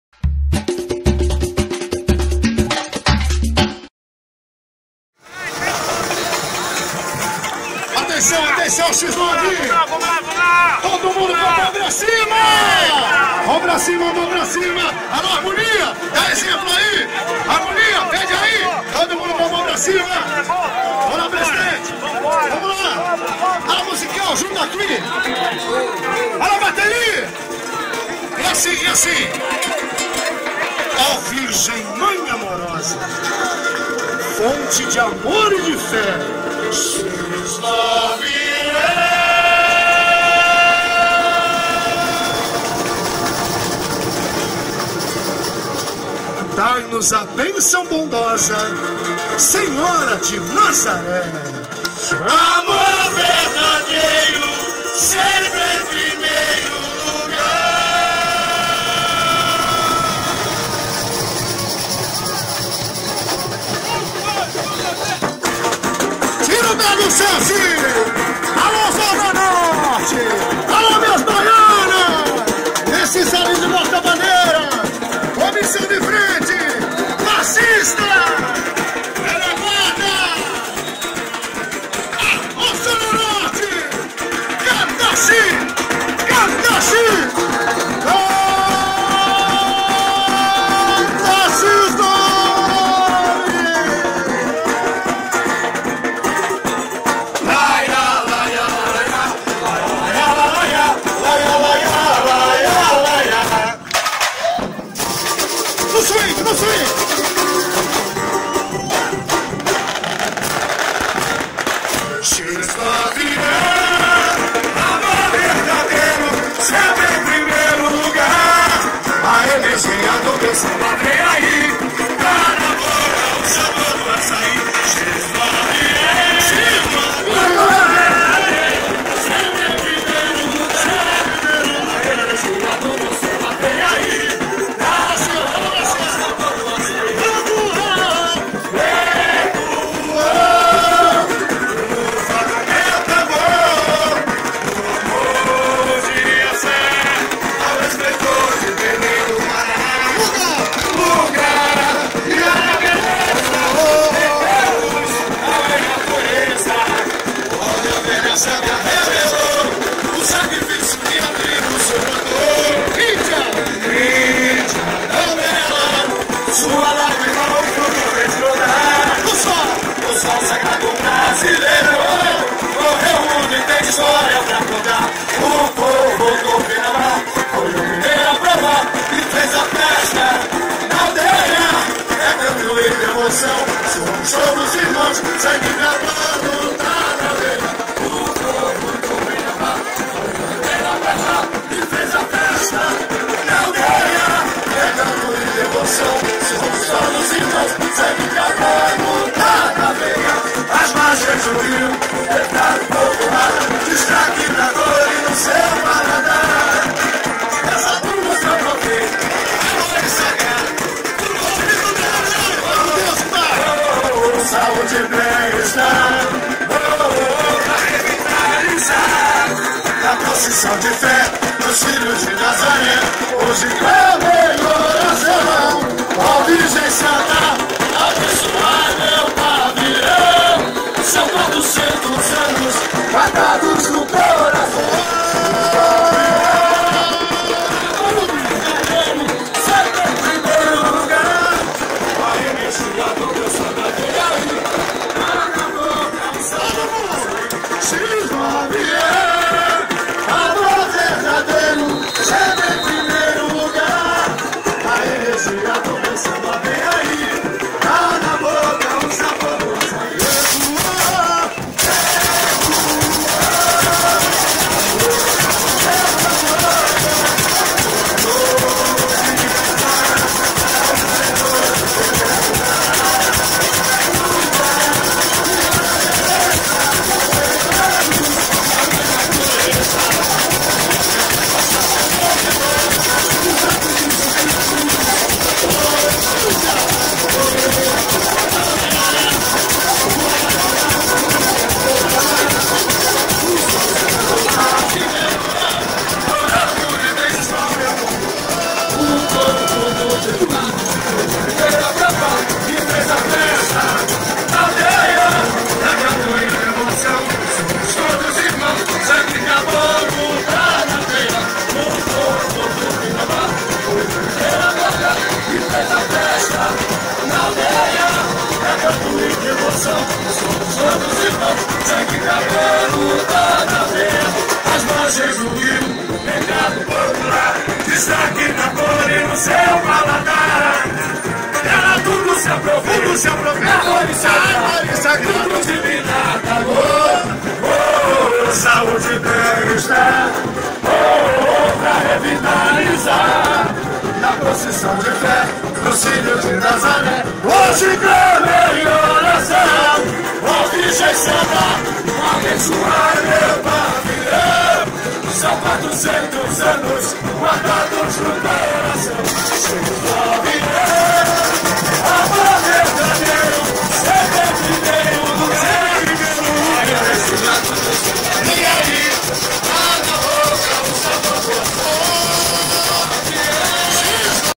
X-9 Paulistana 2016 - Largada 1º Ensaio Técnico | TV SASP
Gravado durante o ensaio técnico da escola realizado em 16/1, no sambódromo do Anhembi!